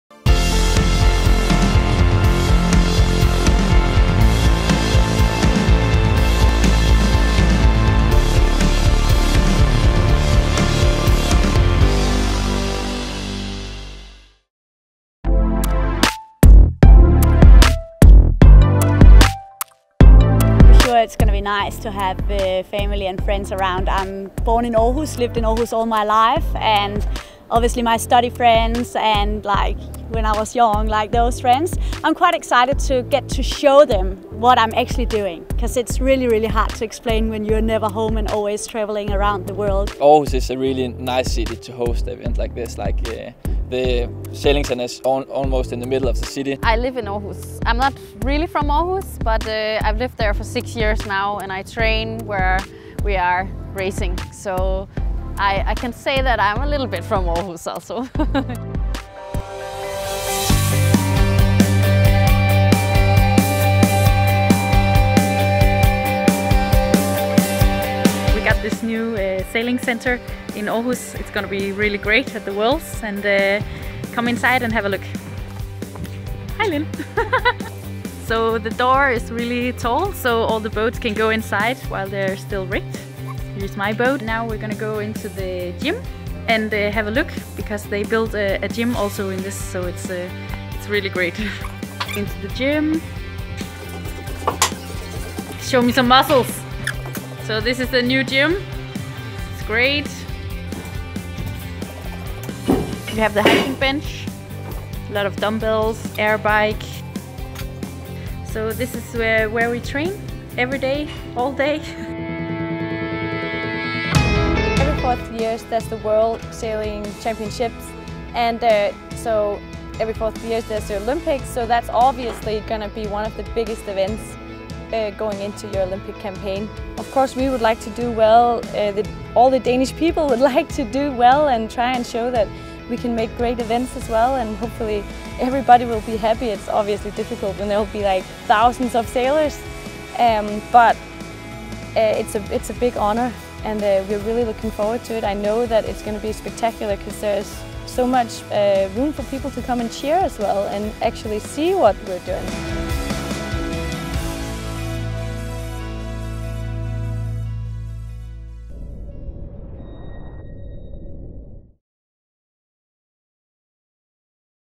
Chasing Dreams | Danes on Home Waters
We asked Christian Peter Lübeck, Lin Ea Cenholt, Anne-Marie Rindom, Jena-Mai Hansen and Katja Salskov Iversen to show us around Aarhus and tell us what it means to be sailing in their home country at the Hempel Sailing World Championships...